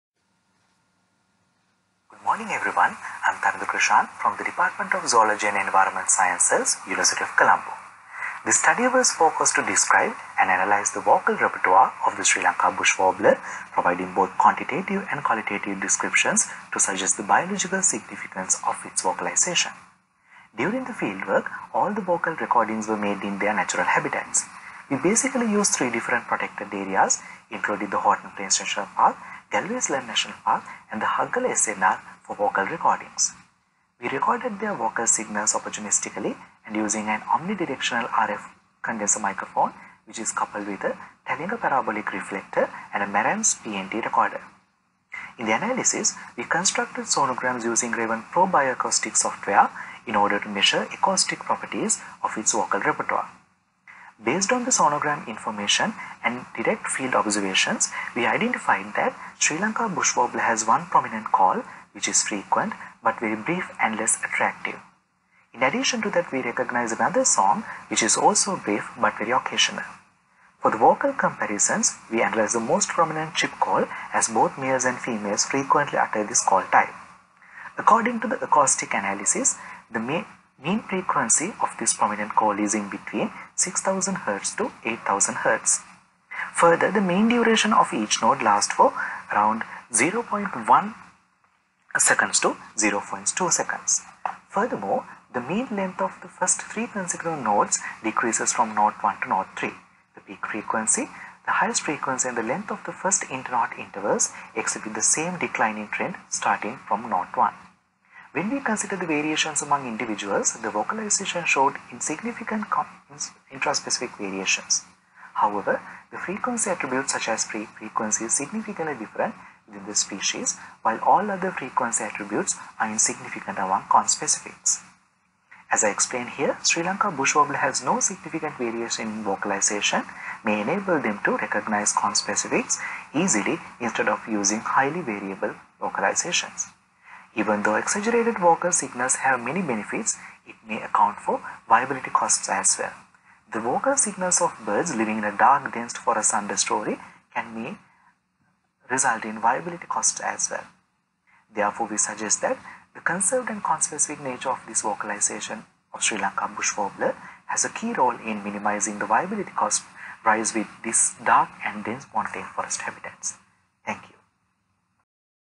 924/D SLAAS 2020 Annual Sessions Poster Presentations - Section D
Inconspicuous vocal repertoire of endemic Sri Lanka Bush Warbler (Elaphrornis Palliseri): A quantitative description of within-species variation in vocalization